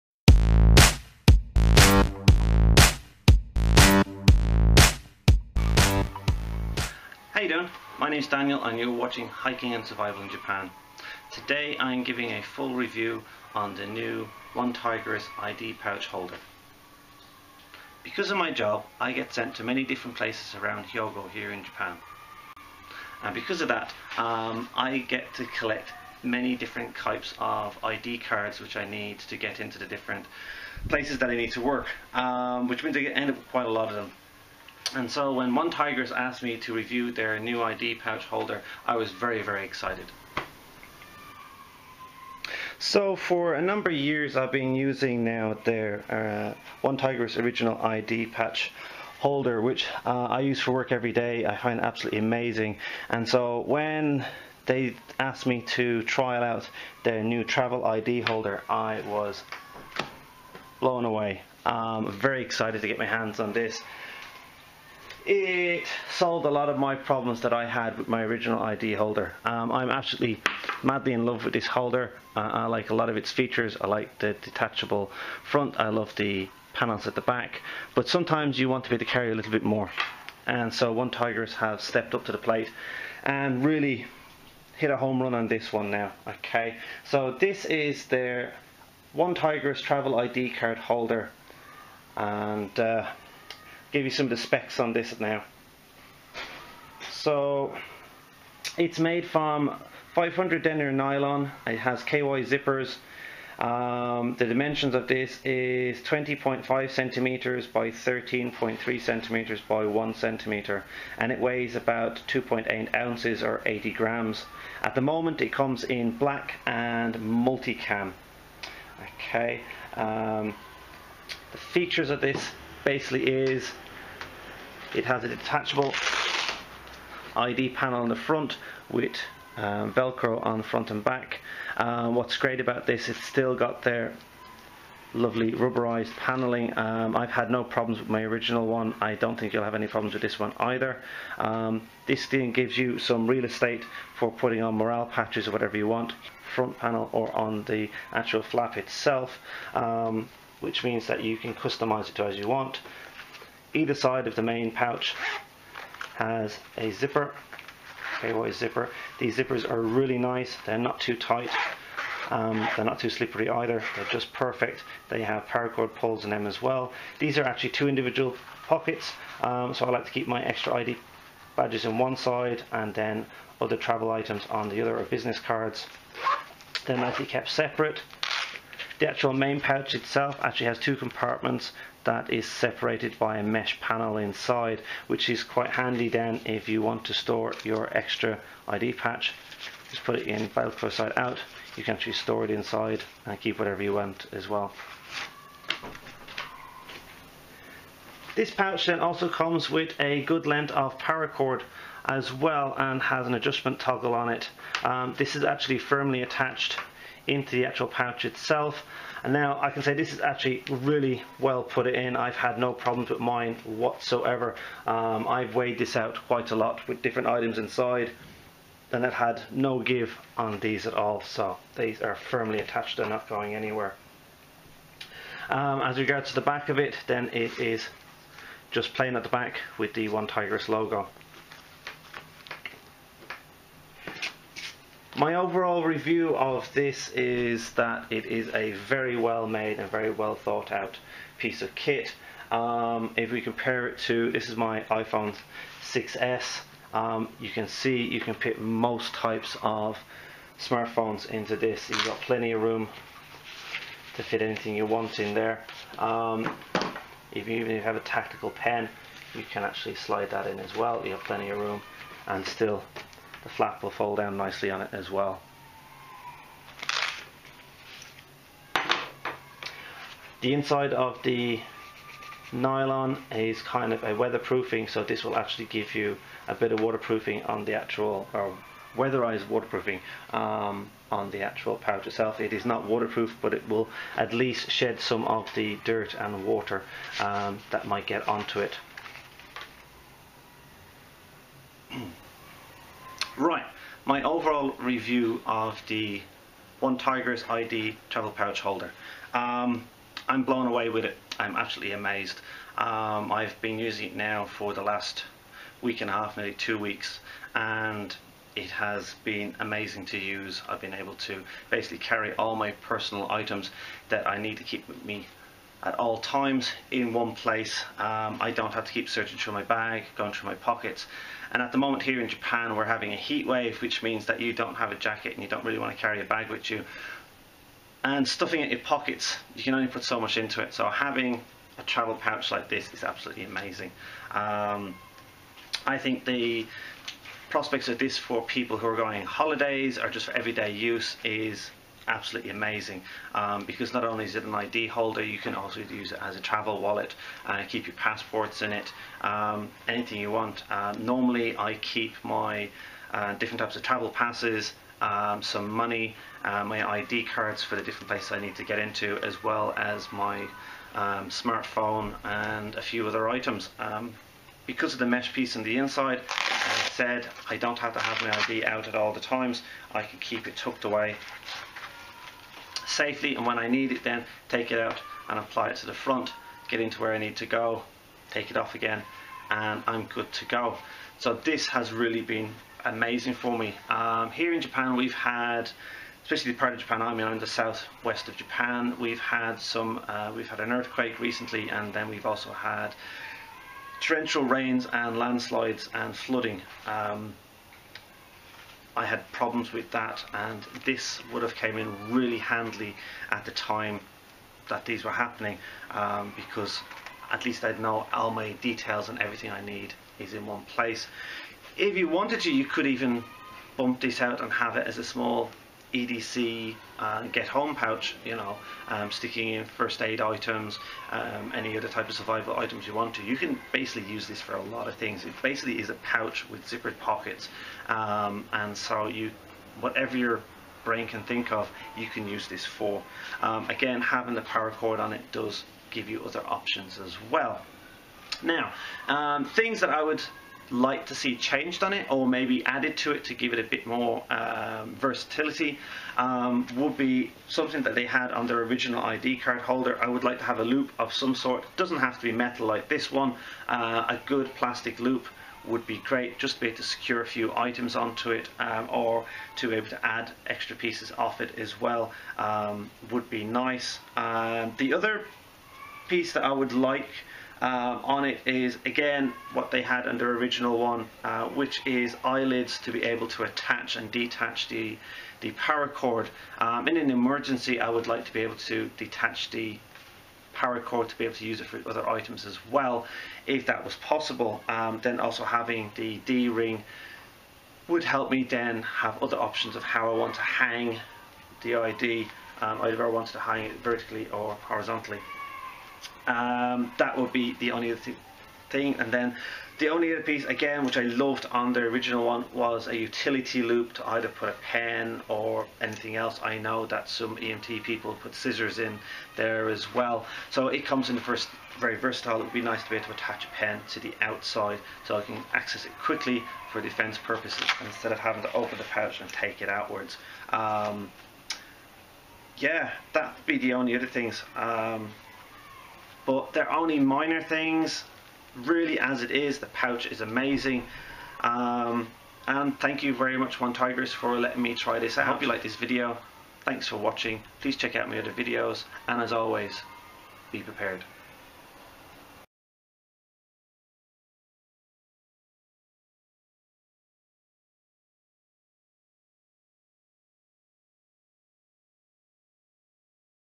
OneTigris ID Card Holder and Travel Wallet with Lanyard
I was asked by OneTigris to try out and give a review on their new product. The OneTigris ID Card Holder and Travel Wallet with Lanyard.

Thank you OneTigris for asking me to review this item.